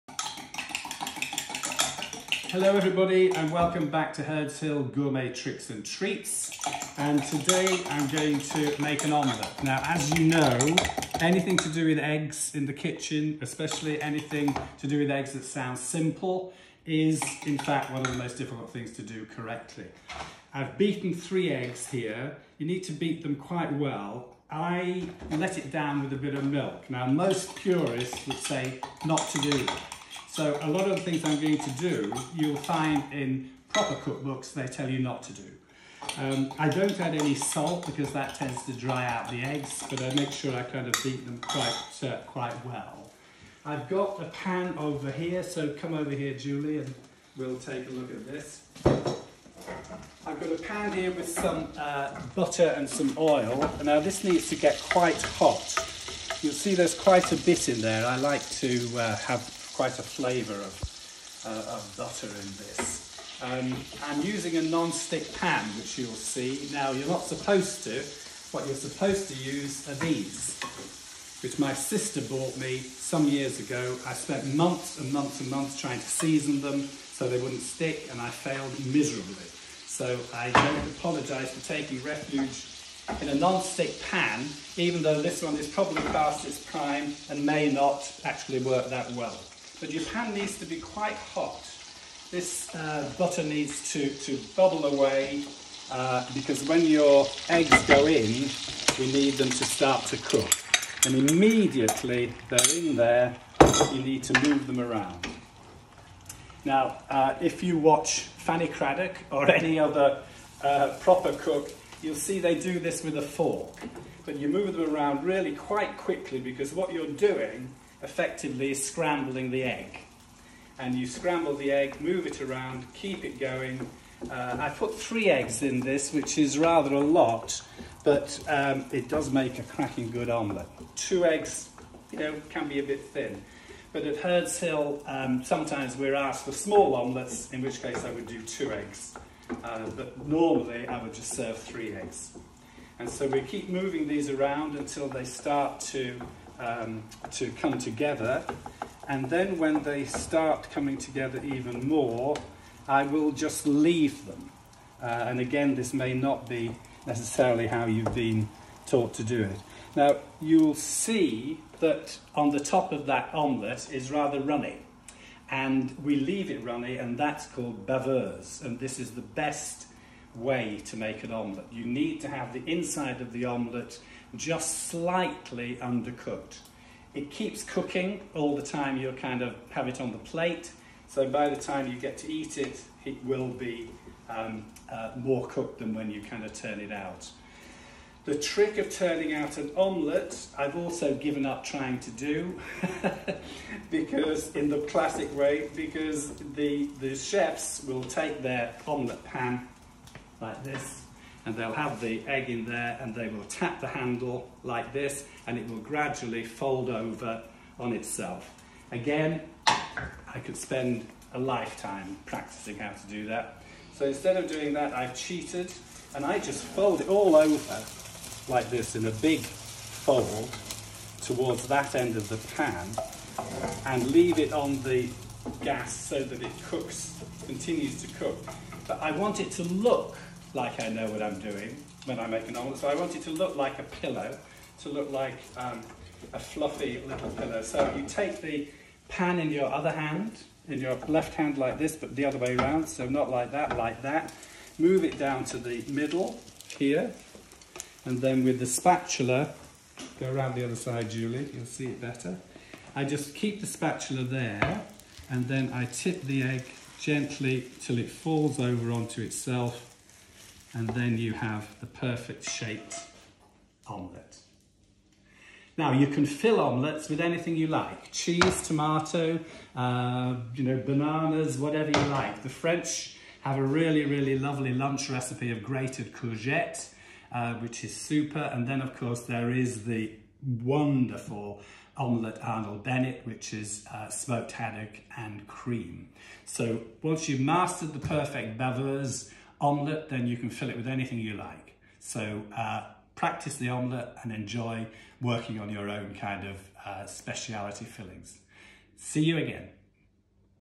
Gourmet Tricks & Treats: The classic three-egg omelette
David Holmes - owner of Hurds Hill in Somerset, shows us top tips and tricks to making the perfect three-egg omelette